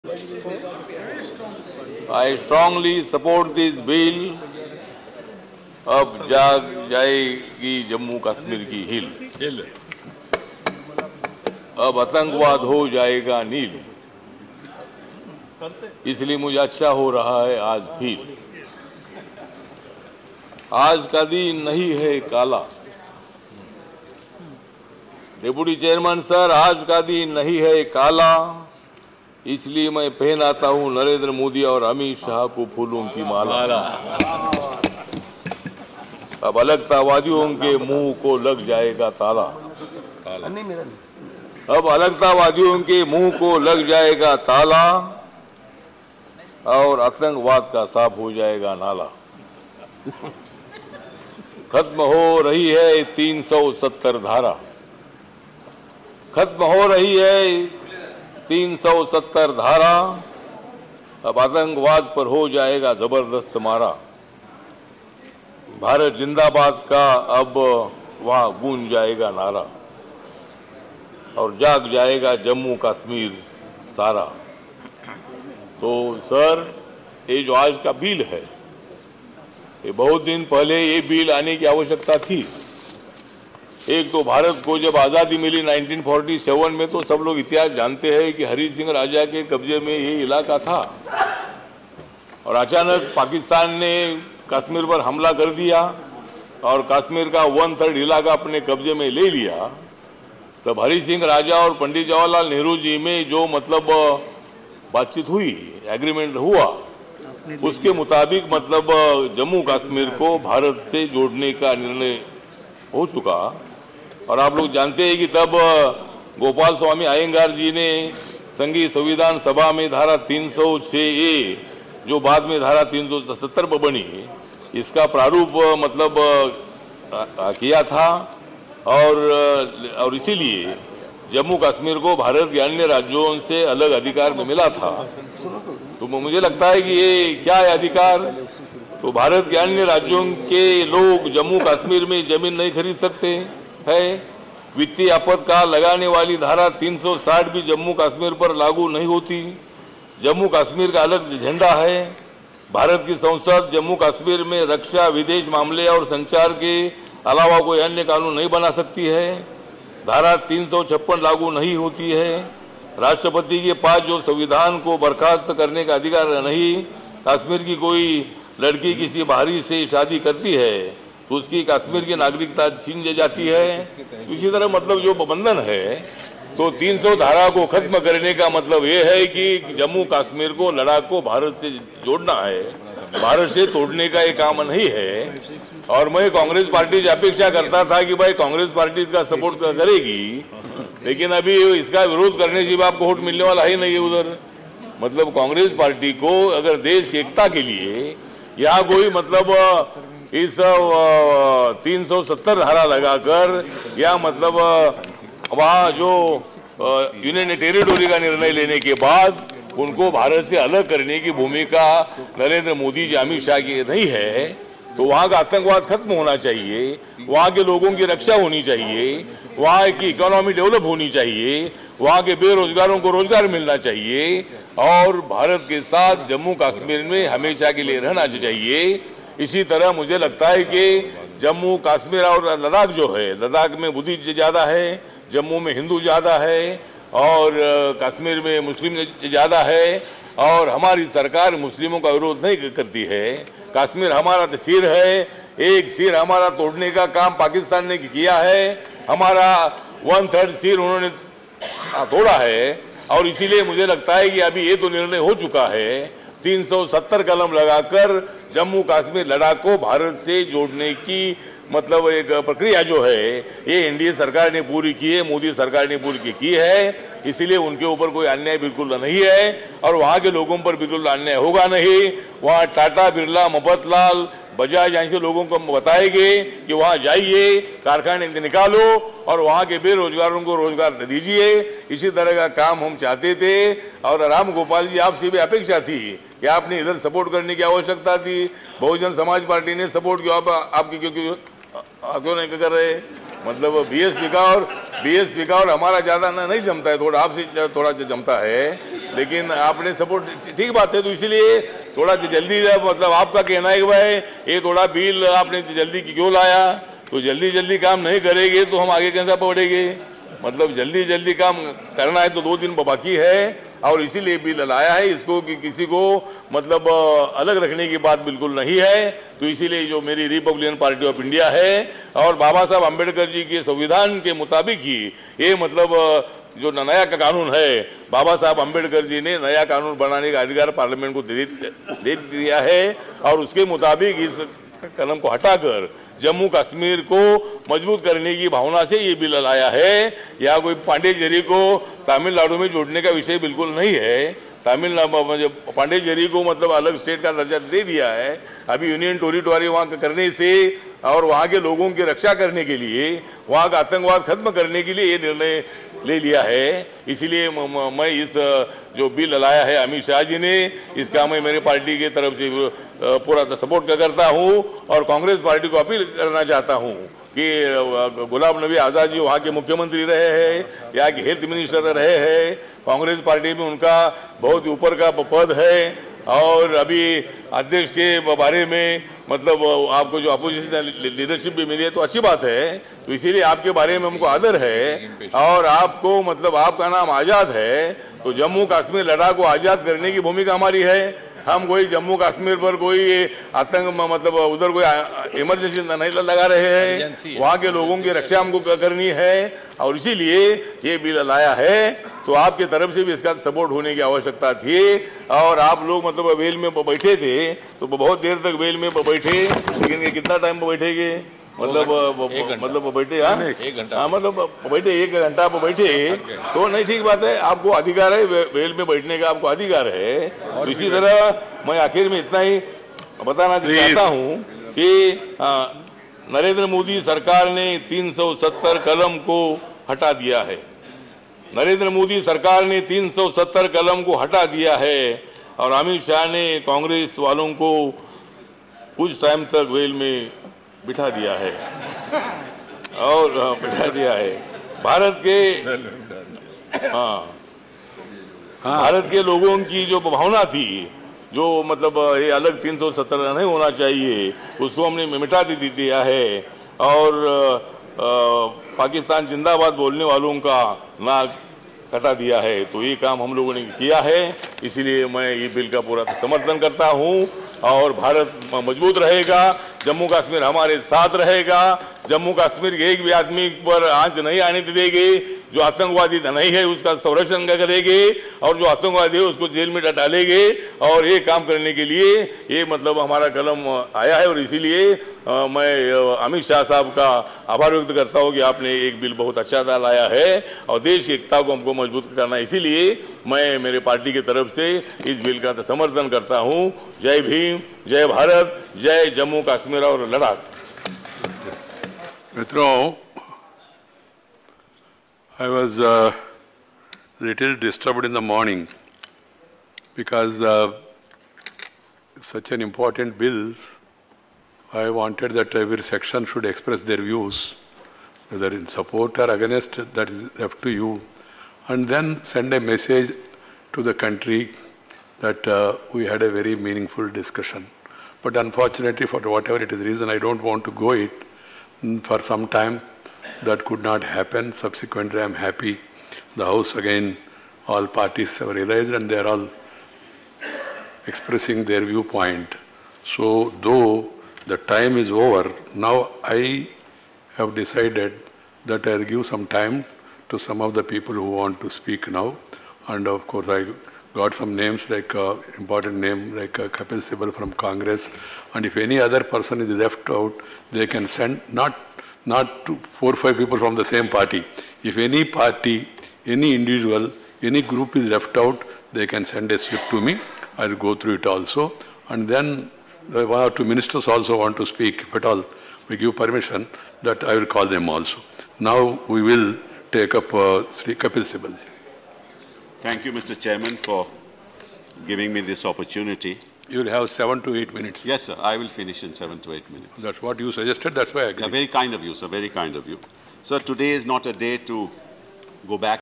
Ramdas Athawale's Excellent Speech On Jammu and Kashmir Reorganisation Bill 2019 | YOYO TV Kannada
Central government has decided to remove Article 370 and split the state into two union territories, taking a major decision on Jammu and Kashmir. Amid opposition from opposition parties, Union Minister of State Ramdas Athawale has supported the government's decision to recite poetry in the Rajya Sabha in his well-known style.

Union Minister of State, Athavale, while reading the poem said, "Today is not black, so I wear Narendra Modi and Amit Shah." Athawale further said, 'I strongly support this bill. It is a welcome decision from the government towards making Jammu and Kashmir free from terrorism, which is supported by my Republican Party of India.

"Now awakening the movement of Jammu and Kashmir, now terrorism will be Nil, so I am feeling good / today is not black, therefore I And Amit Shah, I will wear garlands / separatists' mouths. Now the lock and terrorism will be cleared Nala / Bharat Zindabad will be echoed. Now the slogan will be there and J & K Sara will wake up.

"The Narendra Modi government has removed 370 law and Amit Shah has put Congressmen in the well for some time. We have erased the feelings of the people of India / Pakistan has wiped out the names of those who have spoken."

Athavale said, 'Kashmir is the head of our country. Pakistan has already done the job of breaking our head. This bold step of government will work to eliminate terrorism. This will put terrorists in jail. This work has been done under the constitution made by Babasaheb Ambedkar and the right to make a new law.

Union Minister Athavale has targeted senior Congress leader and leader Opposition Ghulam Nabi Azad to oppose this Bill. Athawale said, 'Ghulam Nabi Azad is former Chief Minister of Jammu and Kashmir and he has also been a minister in the central government. He is a big Congress leader and should have supported the bill for the interest of Kashmir. His name is free and we are liberating Kashmir. Similarly, he also targeted Samajwadi Party leader Ram Gopal Yadav for not supporting the bill.